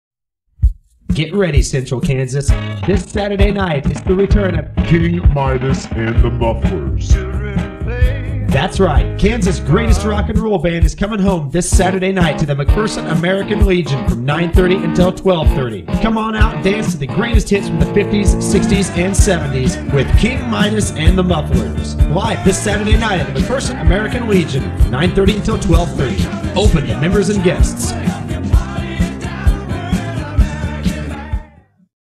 1990s The Lazer 105.9 King Midas and The Mulfers Radio Promo
1990s The Lazer 105.9 Promo. King Midas and The Mulfers Radio Live at the McPherson Kansas American Legion.